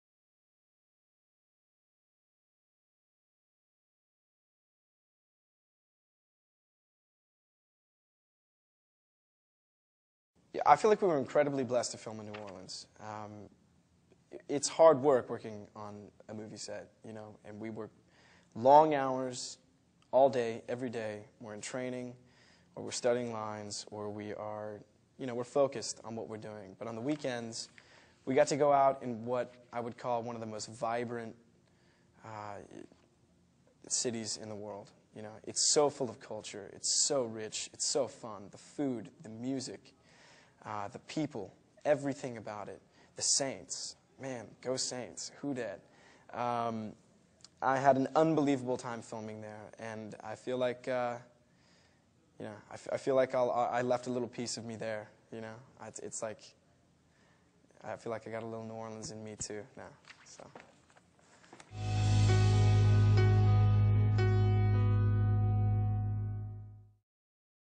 The Lucky One Fan Questions #9 - Le riprese a New Orleans
Cosa ti è piaciuto di più nel filmare a New Orleans? - Risponde Zac Efron (Sottotitoli in italiano)